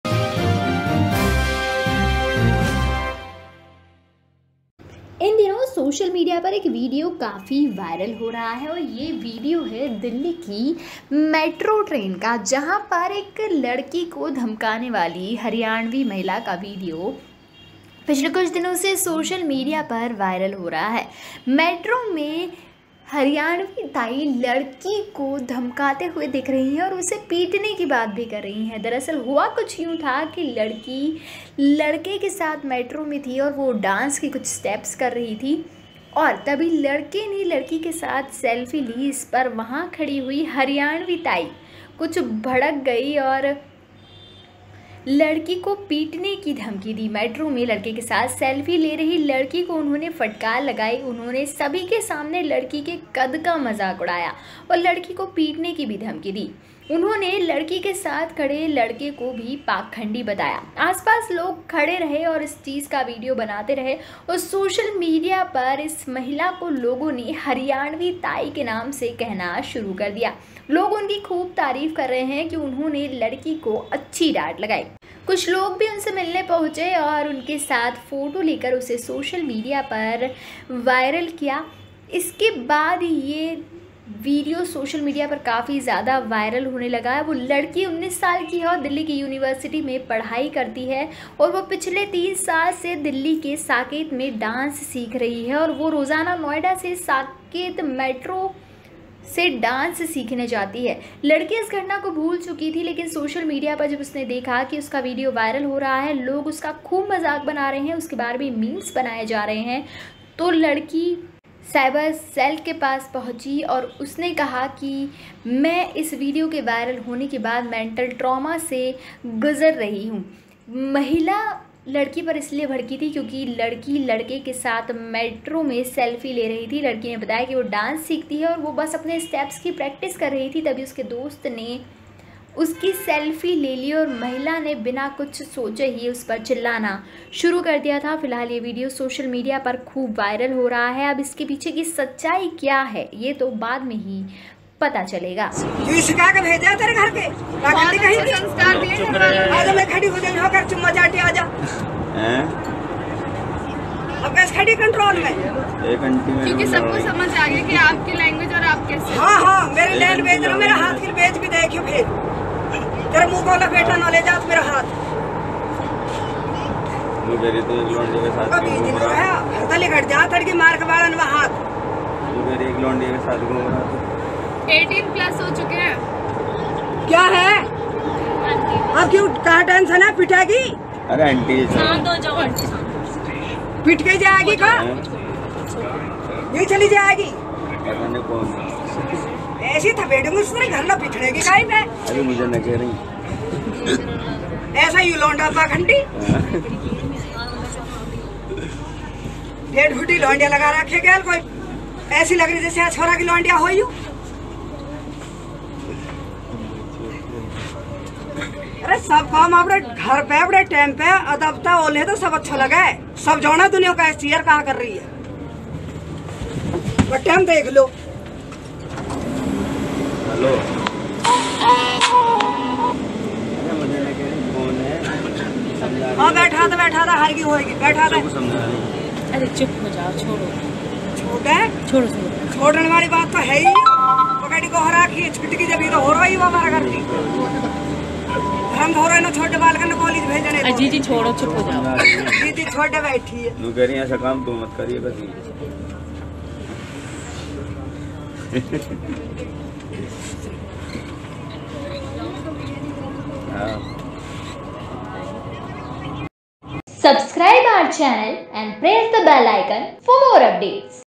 Metro में Kiss कर रहे थे युवक युवती कैसे भड़की ताई | Video Viral | Delhi Metro Train | Ghamasan
क्यों हो रहा है यह वीडियो वायरल |